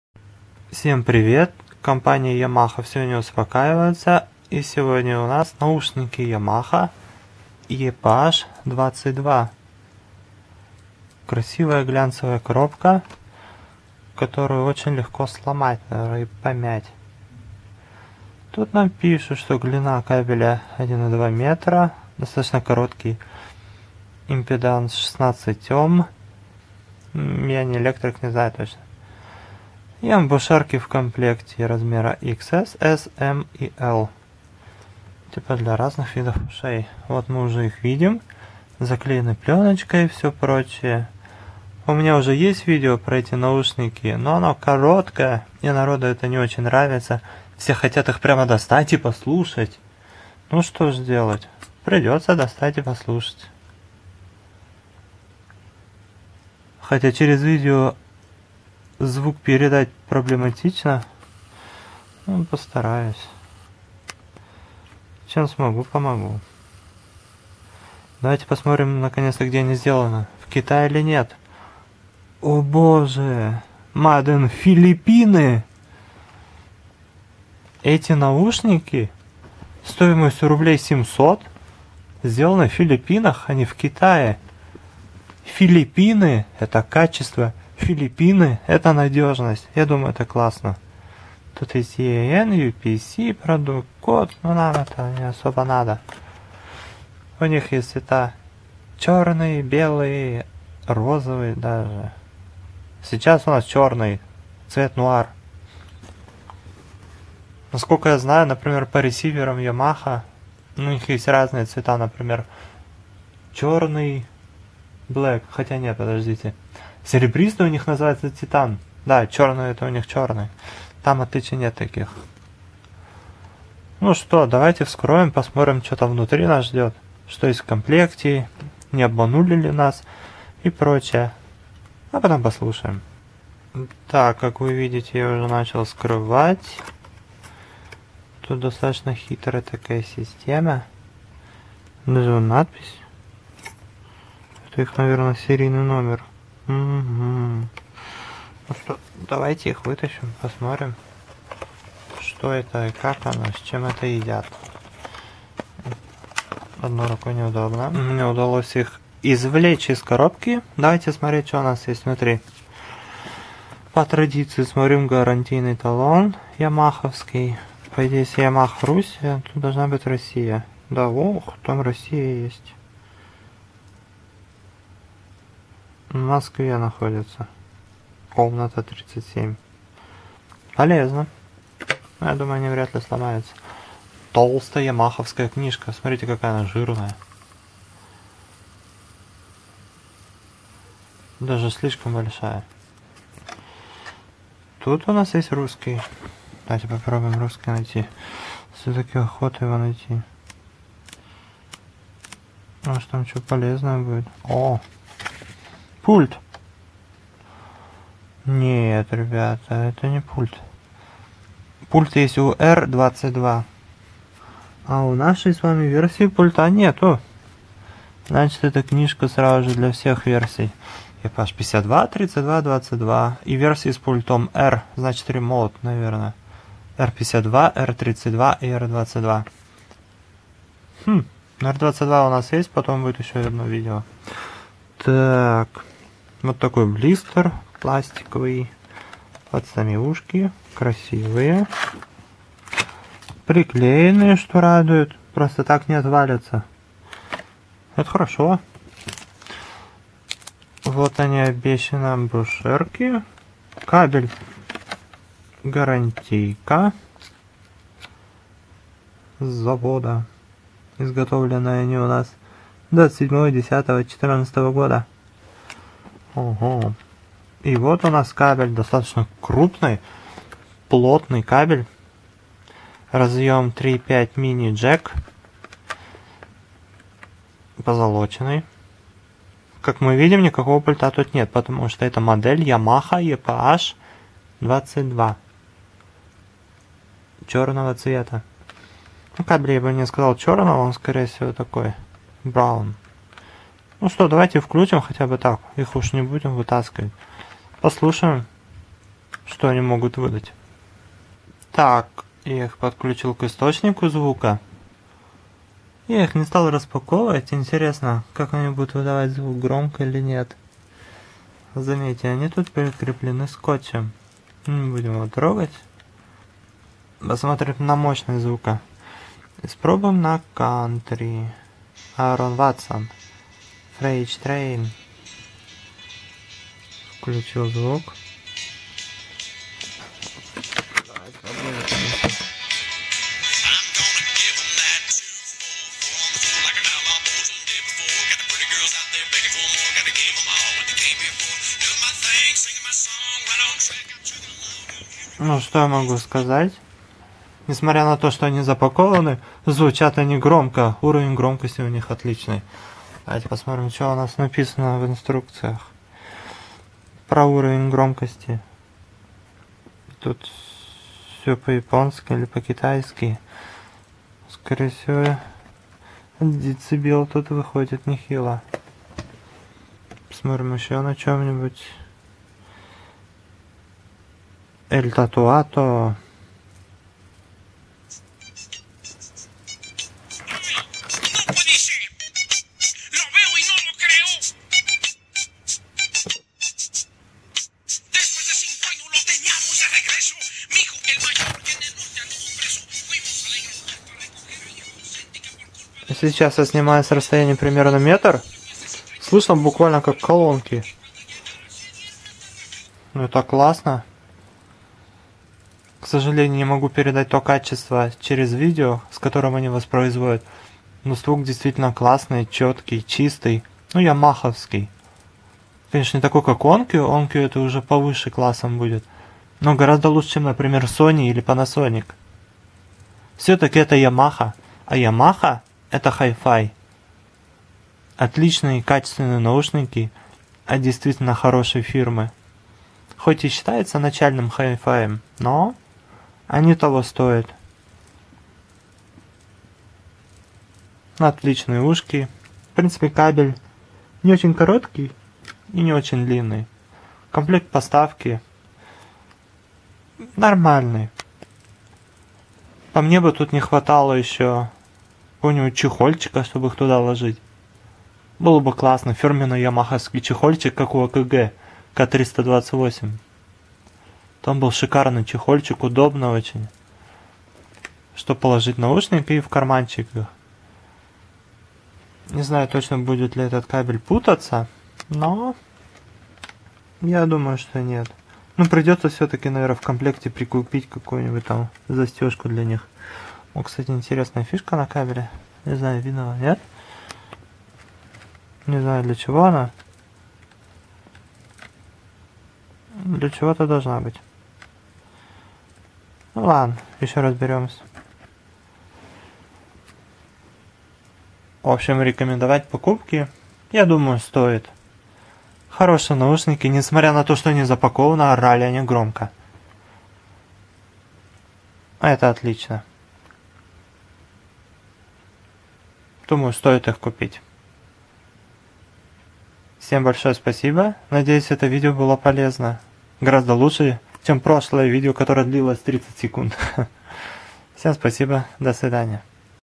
Review Обзор наушников Yamaha EPH-22
Type: closed dynamic
Driver diameter:8.5mm
Impedance: 16 Ω
Output sound pressure level: 99 dB ± 3 dB
Frequency: 20 Hz-40 kHz
:3.5mmL type stereo mini plug